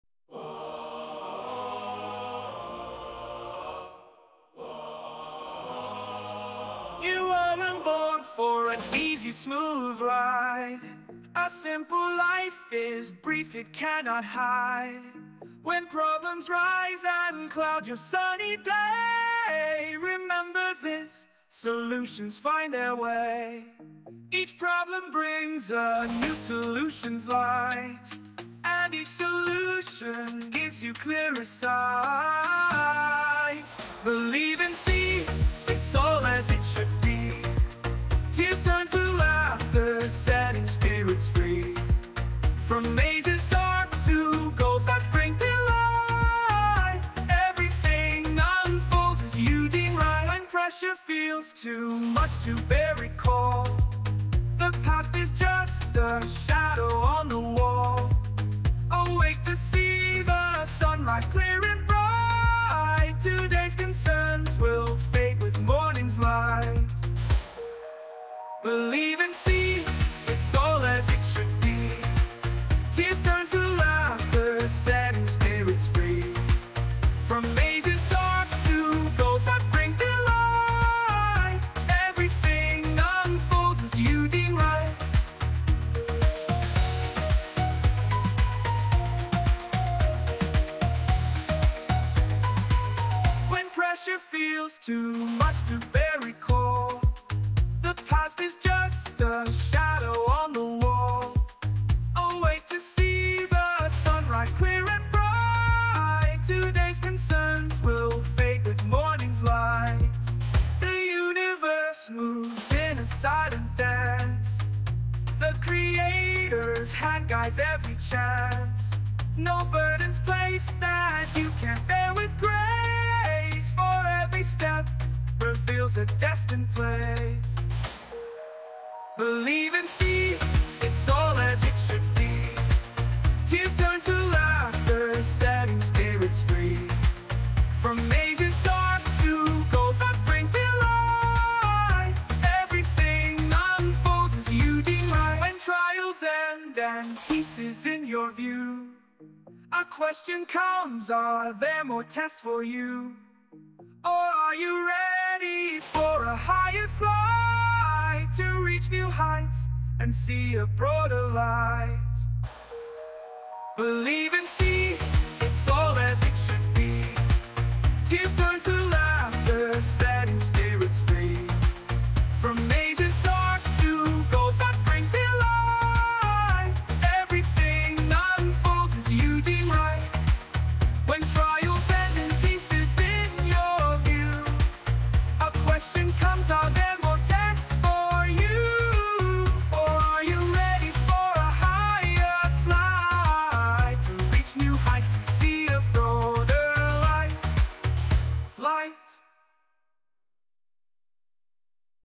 I have poured everything important into this song for you.
I have poured everything important into this song for you. This is the essence; the rest will unfold naturally

(Verse 1)
You weren't born for an easy, smooth ride,
A simple life is brief, it cannot hide.
When problems rise and cloud your sunny day,
Remember this: solutions find their way.
Each problem brings a new solution's light,
And each solution gives you clearer sight.

(Chorus)
Believe and see, it’s all as it should be,
Tears turn to laughter, setting spirits free.
From mazes dark to goals that bring delight,
Everything unfolds as you deem right.

(Verse 2)
When pressure feels too much to bear, recall,
The past is just a shadow on the wall.
Awake to see the sunrise clear and bright,
Today's concerns will fade with morning's light.

(Verse 3)
The universe moves in a silent dance,
The Creator’s hand guides every chance.
No burden's placed that you can't bear with grace,
For every step reveals a destined place.

(Verse 4)
When trials end and peace is in your view,
A question comes: are there more tests for you?
Or are you ready for a higher flight,
To reach new heights and see a broader light?